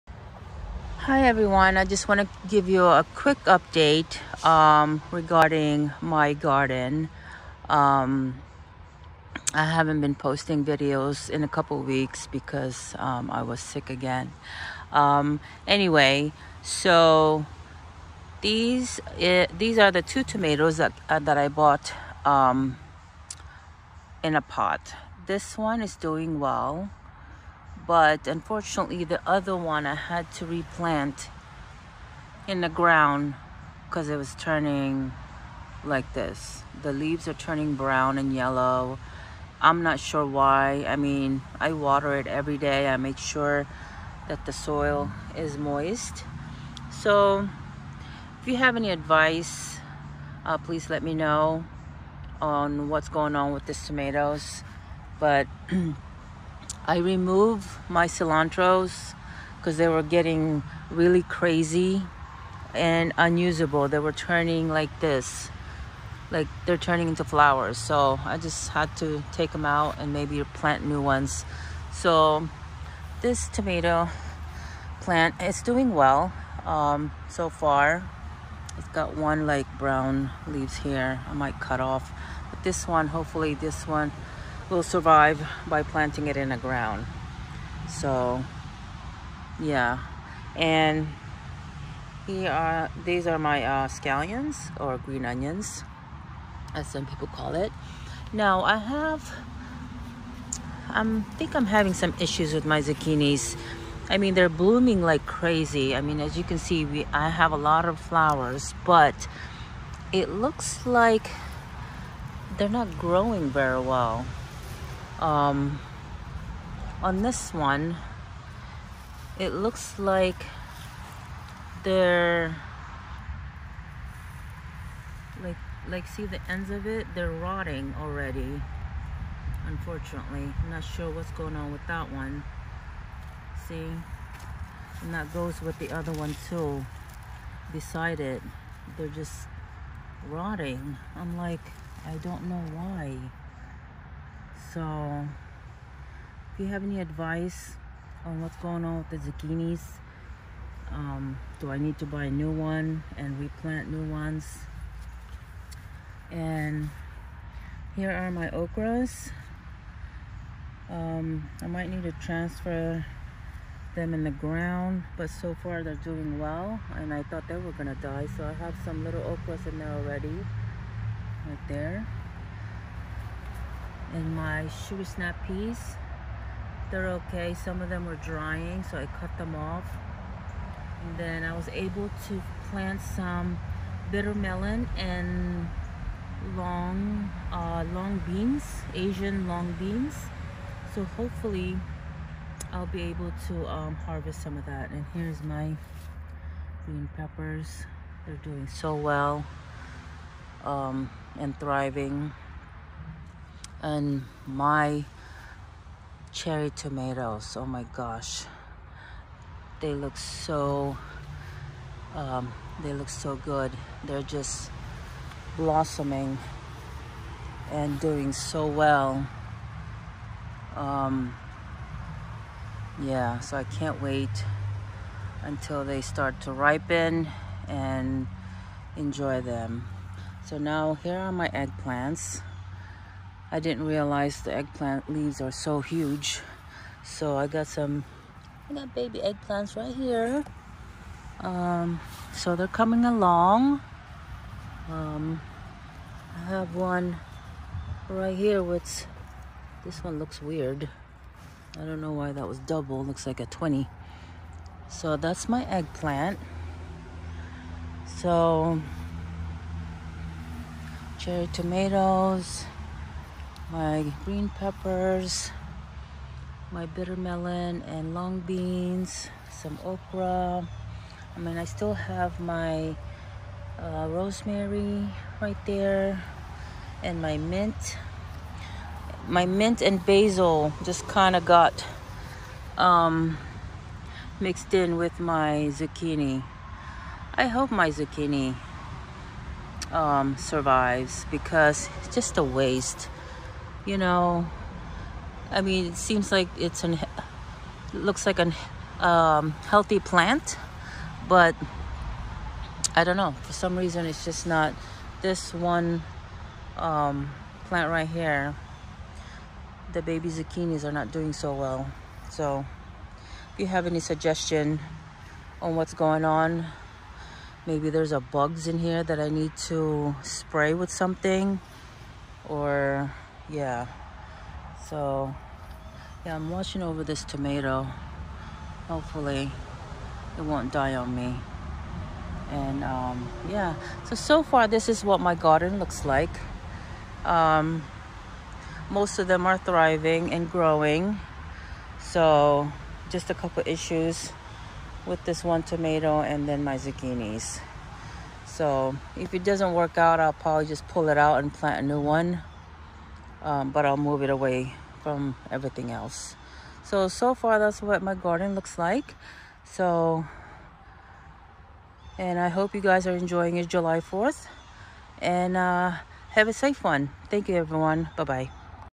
Happy 4th of July🎊🎉- UPDATE on my Garden!!
I have not been uploading videos these past weeks due to an illness.
I just wanted to update you on what's been going on with my garden.